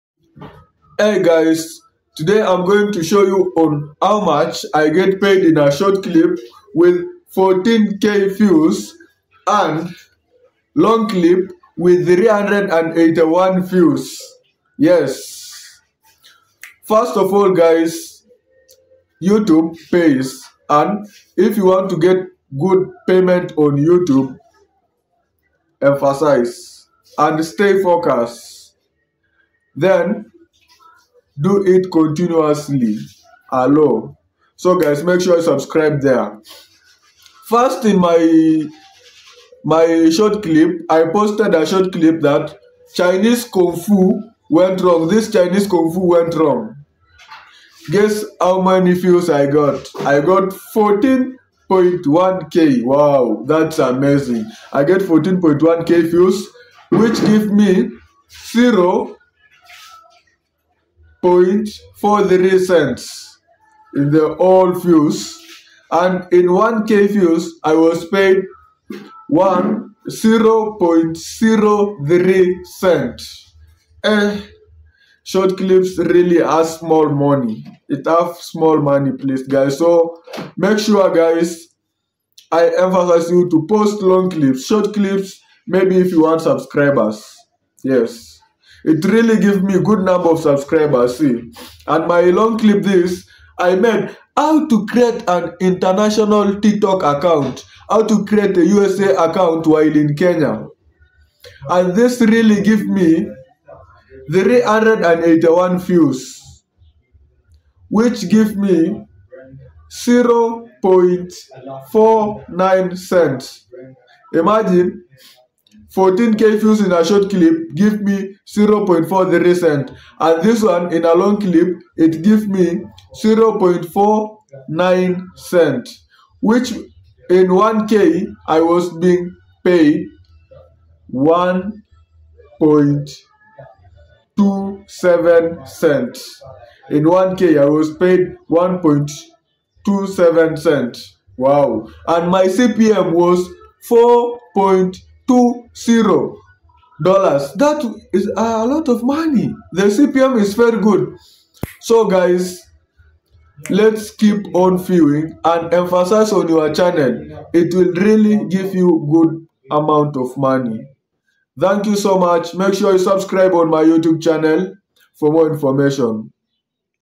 HOW MUCH YOUTUBE PAYS ME THOUGH SHORT CLIPS AND LONG VIDEOS IN 2024
Curious about how earnings differ between short and long videos? Watch this video to learn about my experience with earning potential based on views, comparing a short clip with 14.1k views to a long clip with 381 views. Discover the impact video length can have on ad revenue and engagement! Don't miss out on this insightful comparison."

"Don't miss out on exclusive tips and content on building your online presence and making money on YouTube!